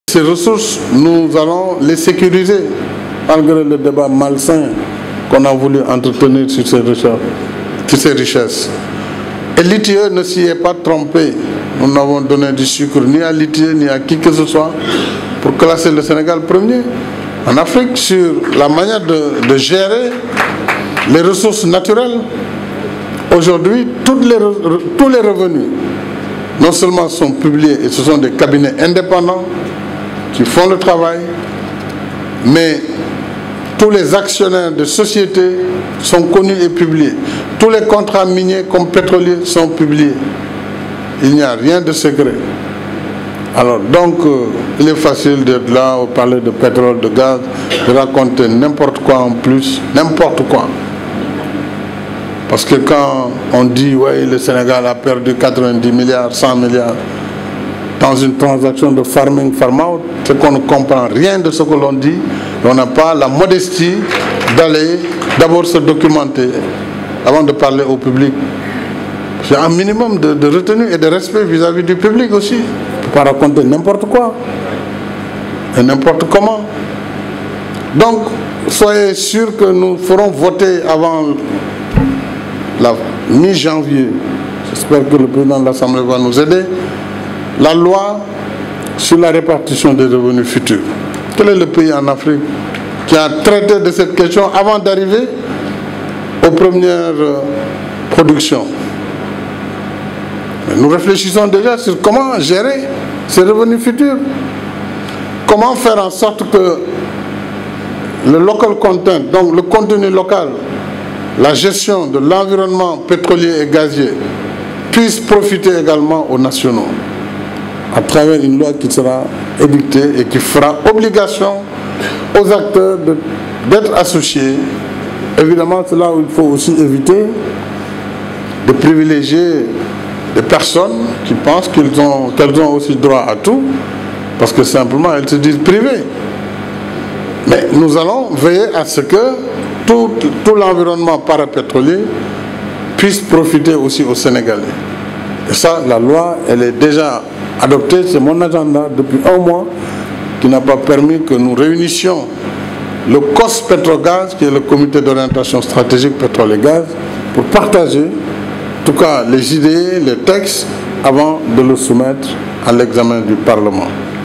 Pétrole et Gaz-Macky se défoule sur l’opposition (Vidéo)
La question relative à la gestion  du pétrole et du gaz s’est invitée à la cérémonie  de  présentation du livre ‘’le Sénégal au cœur’’. Le chef de l’état rassure sur la gestion transparente de ses ressources. Macky Sall renseigne que la loi sur la répartition des revenus futurs pourraient être votée mi-janvier à l’assemblée nationale.